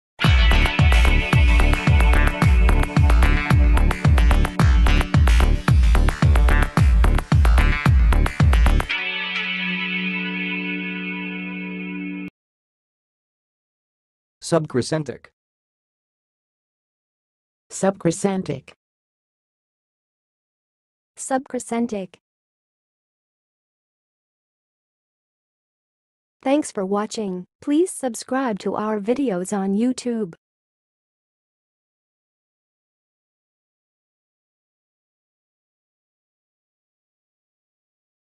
How to say "subcrescentic"! (High Quality Voices)
Watch in this video how to say and pronounce "subcrescentic"!